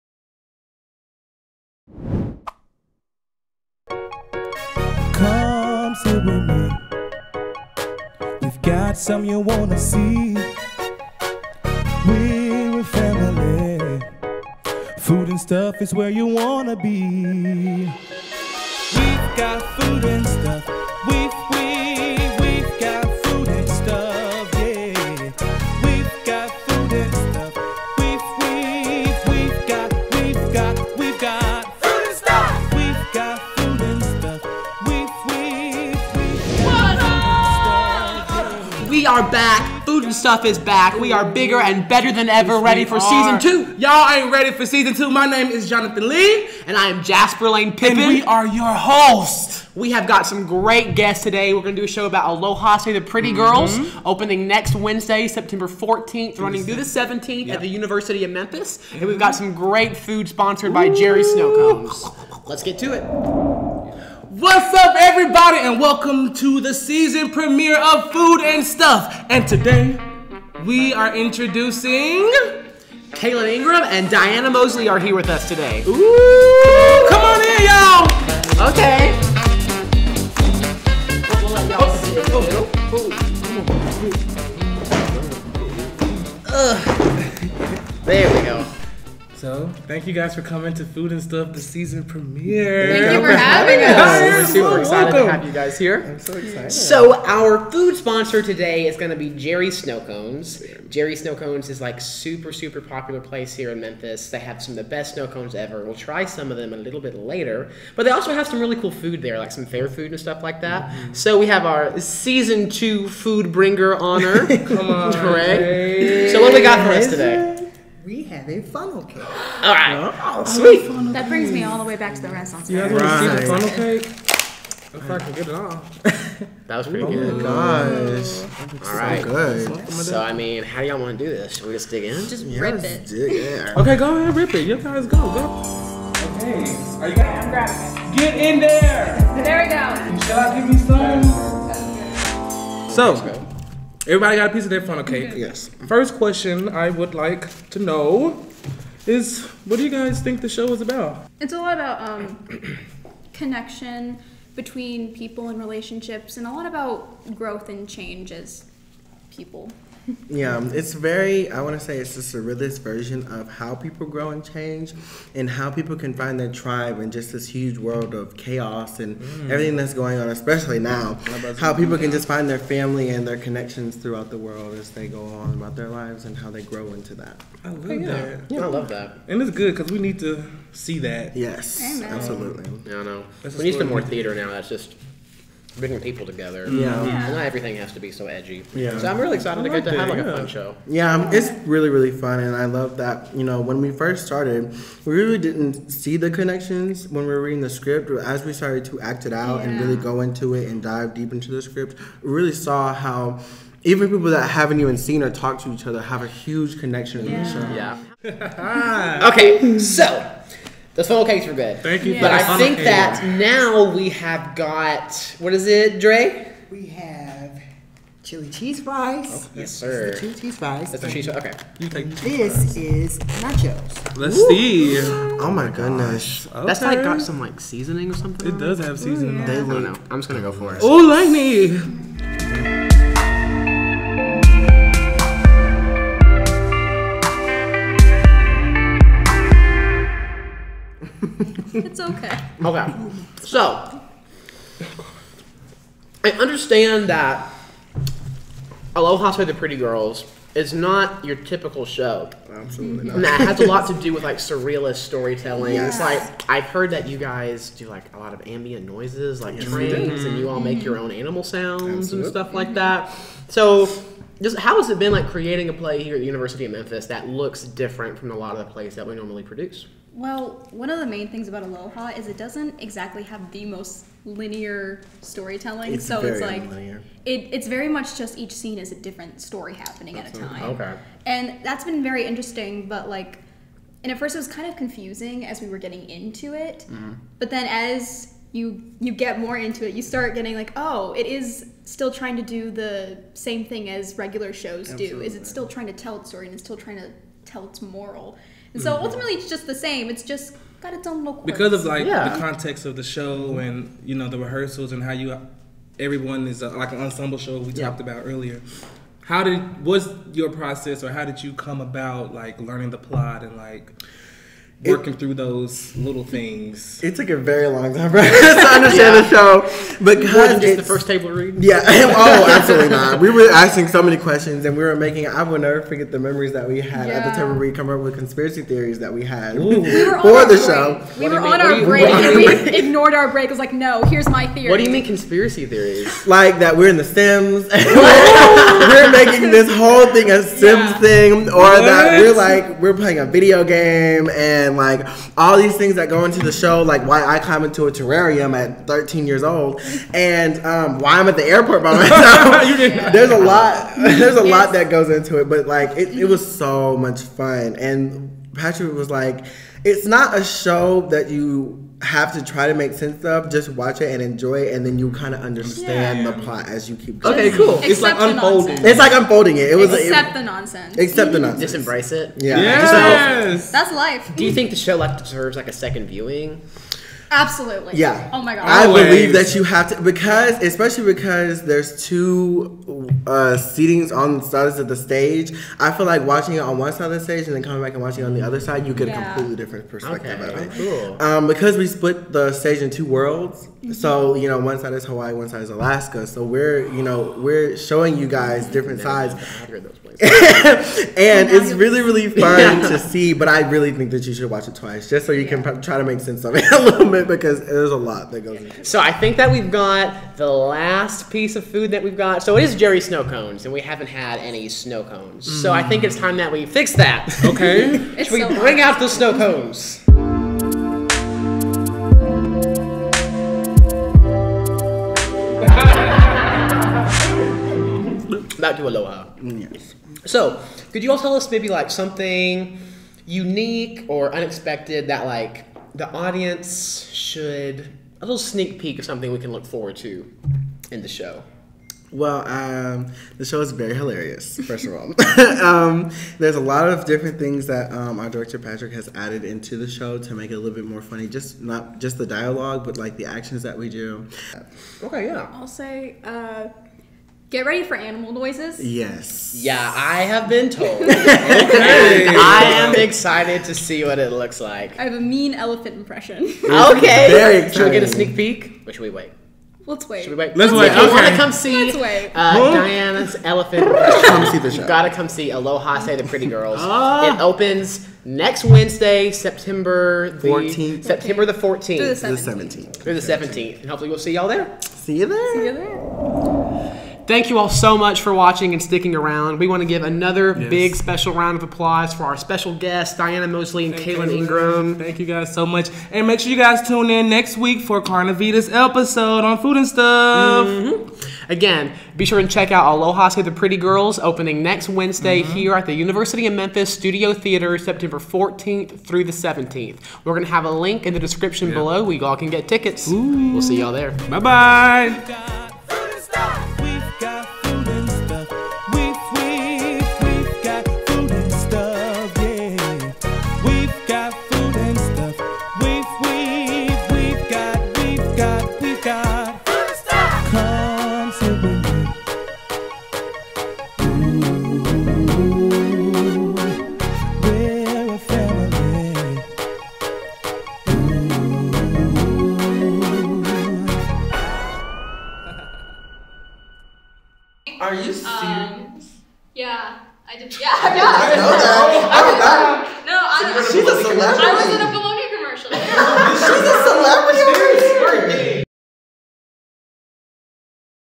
FOOD N' STUFF - Aloha, Say The Pretty Girls
Food N’ Stuff Season 2 PREMIERE!

Watch the Episode to hear about The University of Memphis’ first season show "Aloha, Say the Pretty Girls" by Naomi Iizuka with special guests from the cast Cealan Ingram and Diana Mosley.